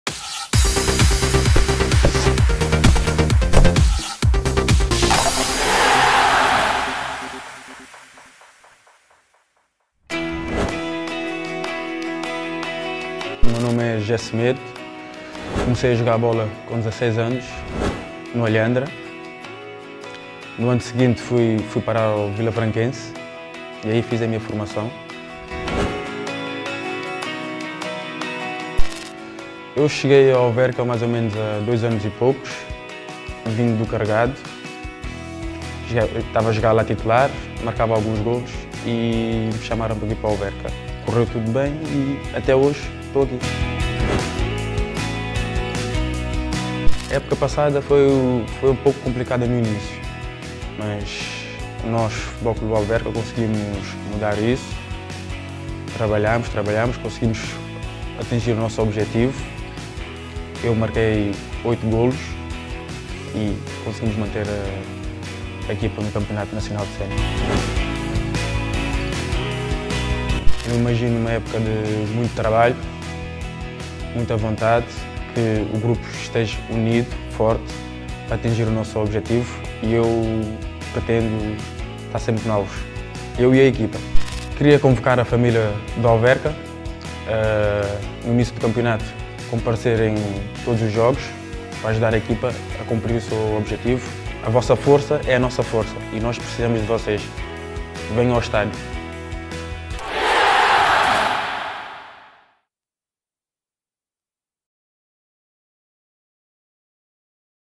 Bastidores com José Semedo
O jogador, natural do concelho, conta-nos um pouco sobre si.